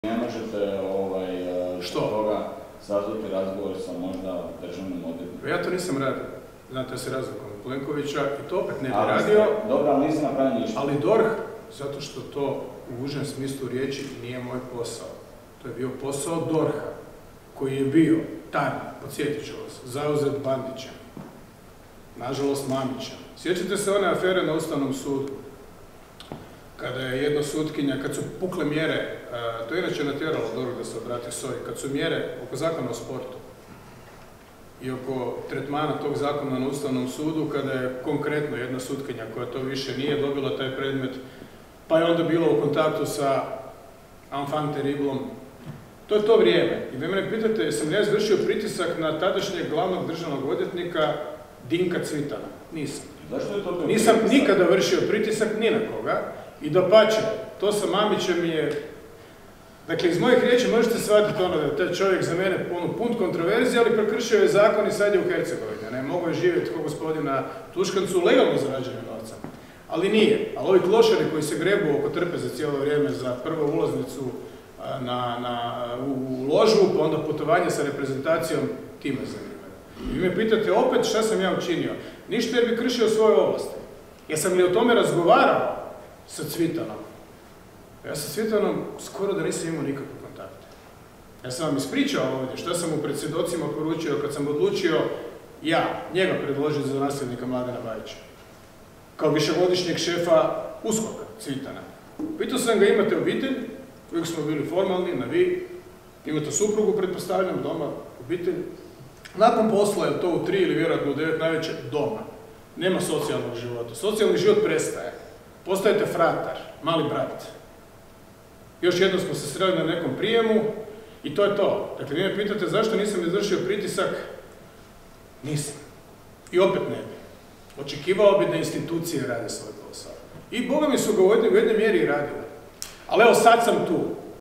Milanović: "Ovi klošari koji se grebu za prvu ulaznicu u loži"
Izvanredna press konferencija predsjednika Milanovića o Ivanu Turudiću.